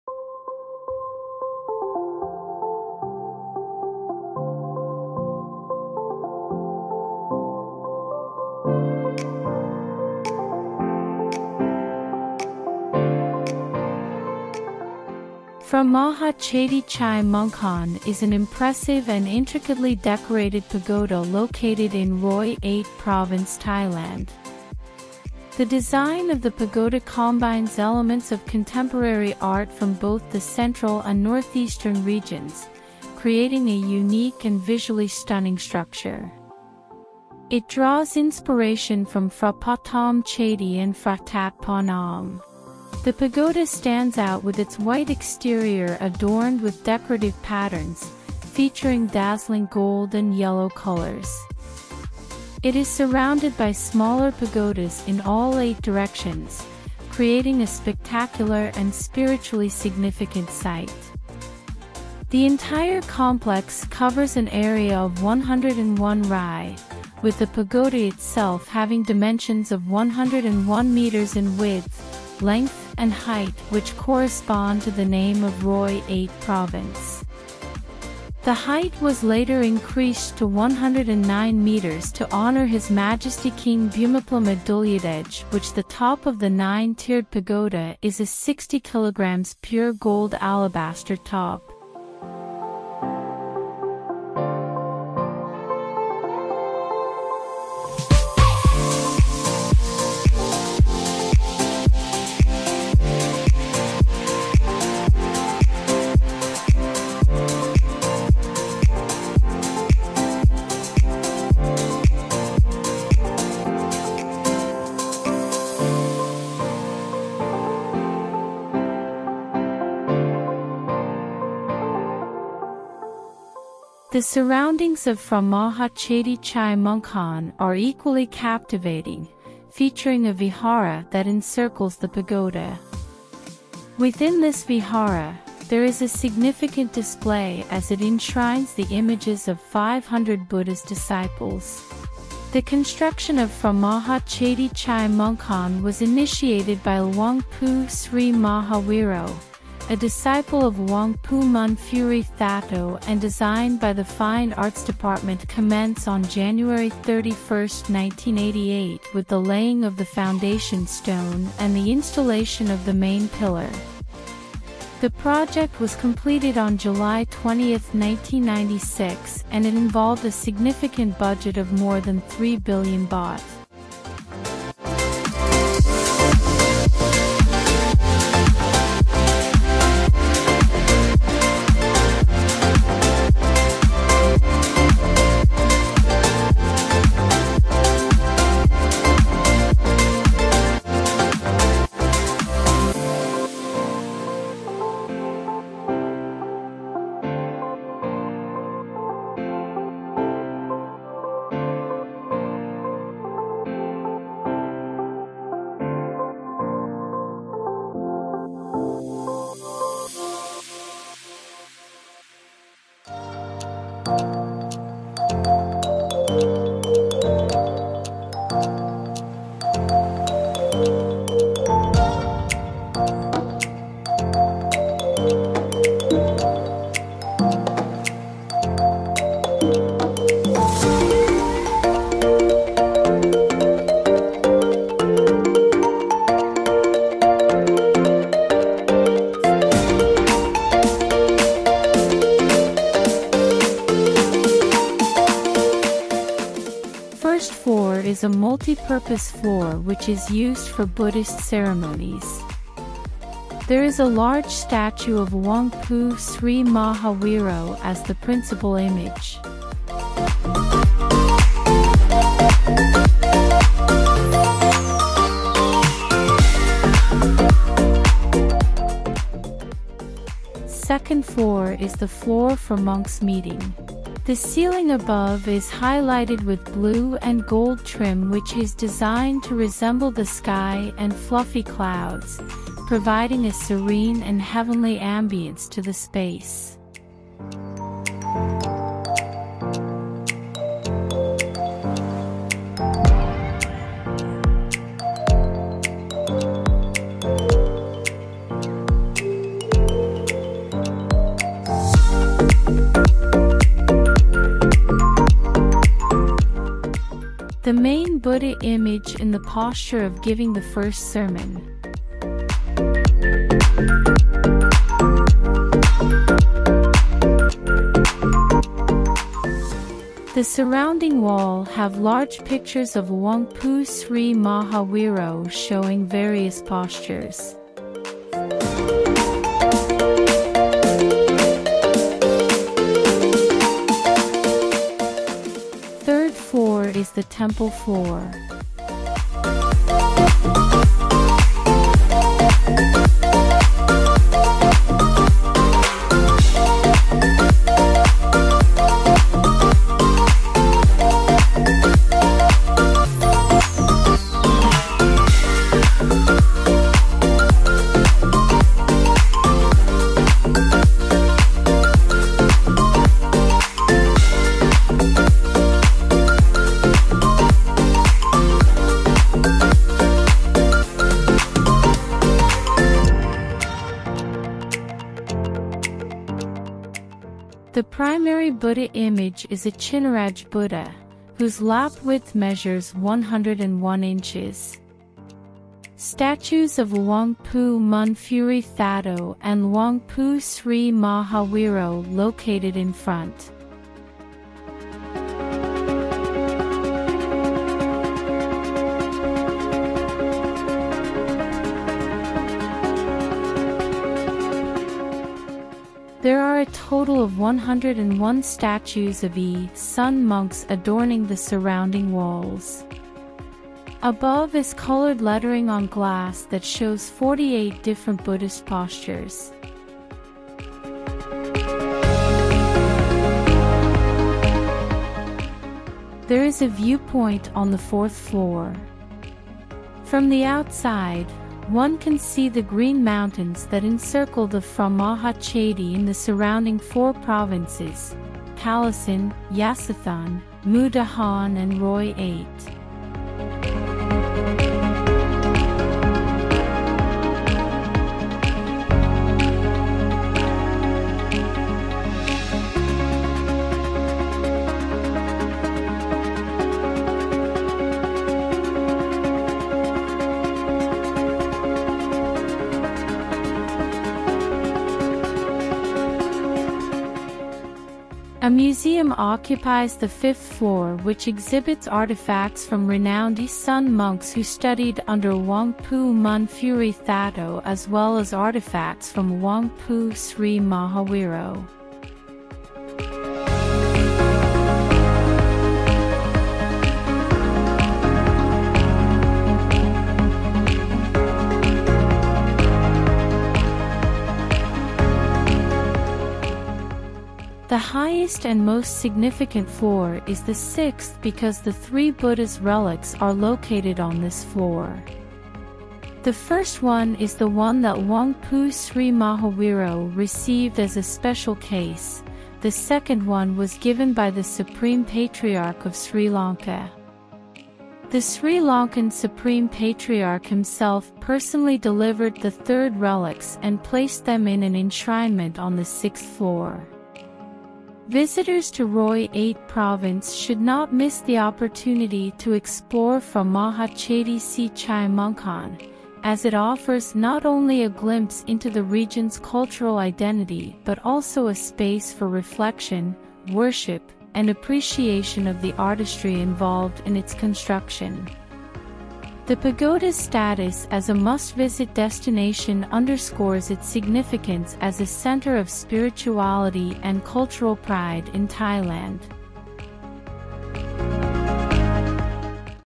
Astonishing 3 Billion Baht Chedi: A Wonder In Roi Et Thailand
Phra Maha Chedi Chai Mongkhon is an impressive and intricately decorated pagoda located in Roi Et Province, Thailand.
The design of the pagoda combines elements of contemporary art from both the central and northeastern regions, creating a unique and visually stunning structure. It draws inspiration from Phra Pathom Chedi and Phra That Phanom.
The pagoda stands out with its white exterior adorned with decorative patterns, featuring dazzling gold and yellow colors. It is surrounded by smaller pagodas in all eight directions, creating a spectacular and spiritually significant site.
The entire complex covers an area of 101 rai, with the pagoda itself having dimensions of 101 meters in width, length and height which correspond to the name of Roi Et Province.
The height was later increased to 109 meters to honor His Majesty King Bhumibol Adulyadej which the top of the 9-tiered pagoda is a 60 kg pure gold alabaster top.

00:00 Introduction
01:35 Chedi surrounding area
03:33 Ist Floor
04:30 2nd Floor
05:36 3rd Floor
07:08 4th Floor
07:47 5th Floor
08:22 6th Floor

The surroundings of Phra Maha Chedi Chai Mongkhon are equally captivating, featuring a vihara that encircles the pagoda.
Within this vihara, there is a significant display as it enshrines the images of 500 Buddha's disciples.
The construction of Phra Maha Chedi Chai Mongkhon was initiated by Luang Pu Sri Maha Weero, a disciple of Luang Pu Mun Phuri Thatton and designed by the Fine Arts Department commenced on January 31, 1988 with the laying of the foundation stone and the installation of the main pillar.
The project was completed on July 20, 1996 and it involved a significant budget of more than 3 billion baht.
- 1st floor is a multi-purpose floor which is used for Buddhist ceremonies.
There is a large statue of Luang Pu Sri Maha Weero as the principal image.
- 2nd floor is the floor for monks meeting. The main Buddha image in the posture of giving the first sermon. The surrounding wall have large pictures of Luang Pu Sri Maha Weero showing various postures.
- 3rd floor is the temple floor. The primary Buddha image is a Chinnaraj Buddha, whose lap width measures 101 inches. There are a total of 101 statues of Isan monks adorning the surrounding walls. Above is coloured lettering on glass that shows 48 different Buddhist postures.
- 4th floor is a viewpoint. From the outside, one can see the green mountains that encircle the Phra Maha Chedi and surrounding four provinces: Kalasin, Yasothon, Mukdahan and Roi Et.
- 5th floor is a museum which exhibits artifacts from renowned Isan monks who studied under Luang Pu Man Phuri Thatto as well as artifacts from Luang Pu Sri Maha Weero.
- 6th floor is the highest and most significant floor because the 3 Buddha's relics are located on this floor.